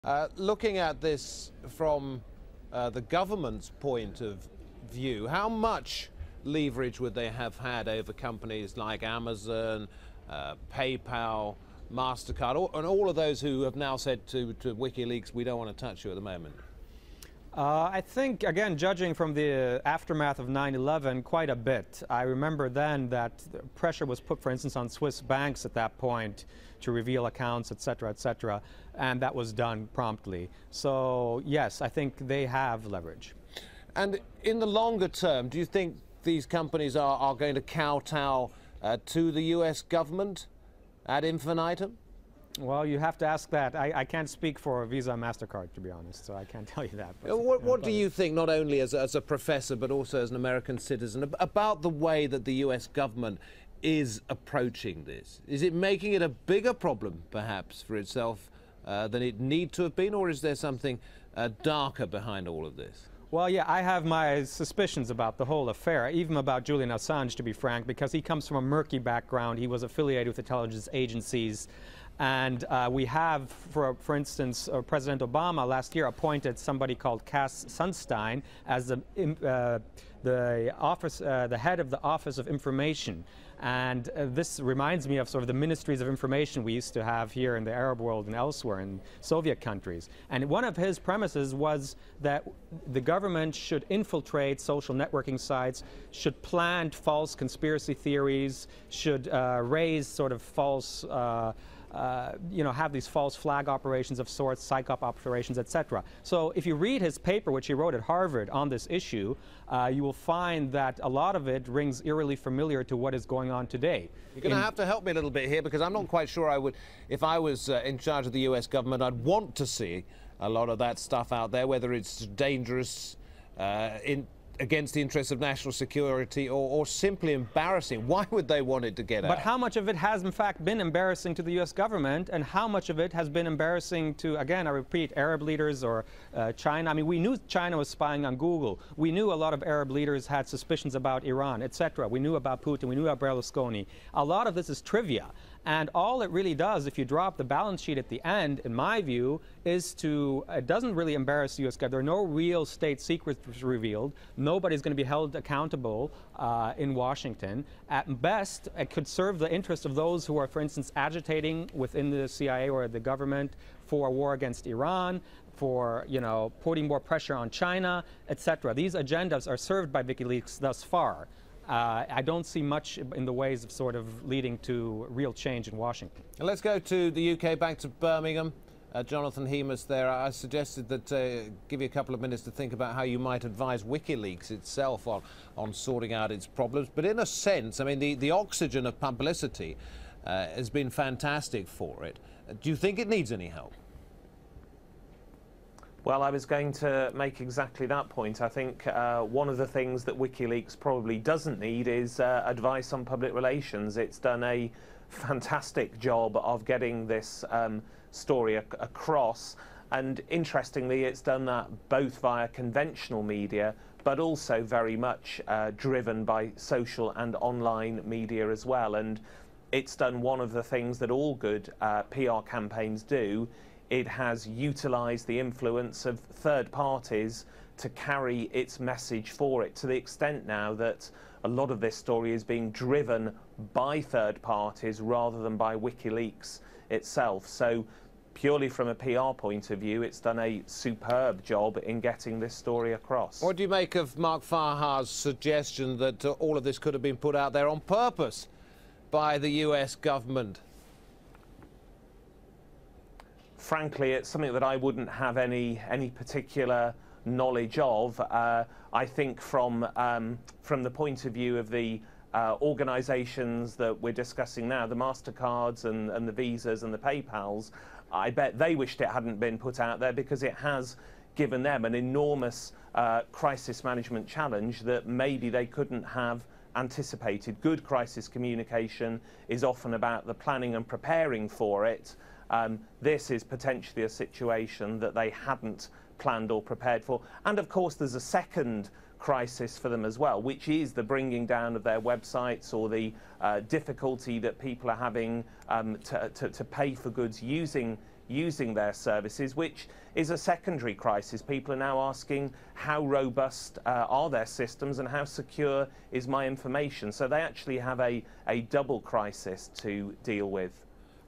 Is "Cablegate" a CIA Honeytrap to Bring Down Wikileaks?
The disinformation war continues... has wikileaks been set up with the US cables to bring down the site and its founder Julian Assange because as Prof. Mark Farha points out no real damage has been suffered by the US government over this particular leak, but it has made Assange some pretty serious enemies around the world, most notably China and Russia.